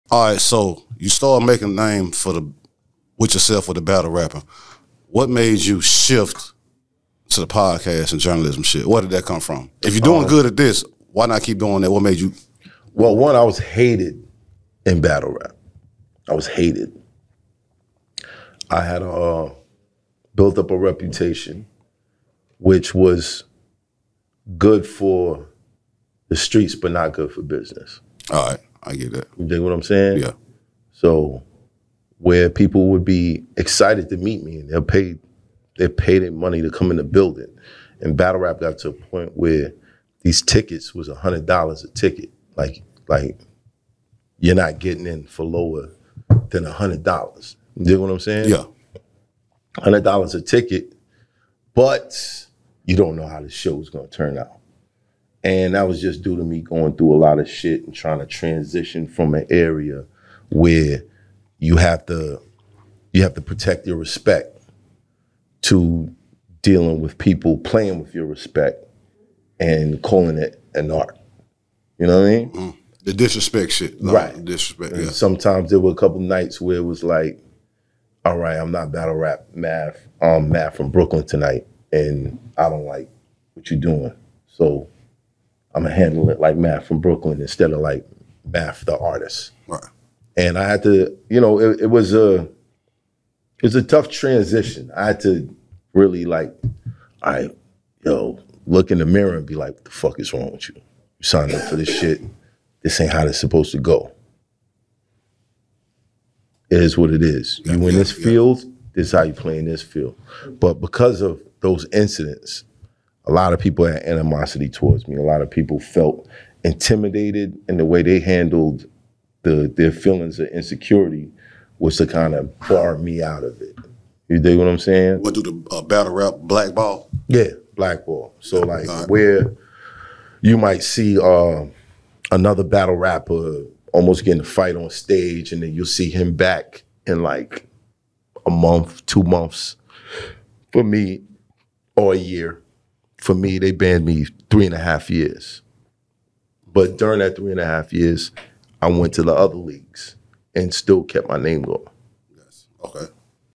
MATH HOFFA- "I was Hated in battle rap!!They banned me so, I started my podcast"
MATH HOFFA talks about how he was blackballed from battle rap, which led him to start "My Expert Opinion" podcast

-follow @maalthepimp on IG & twitter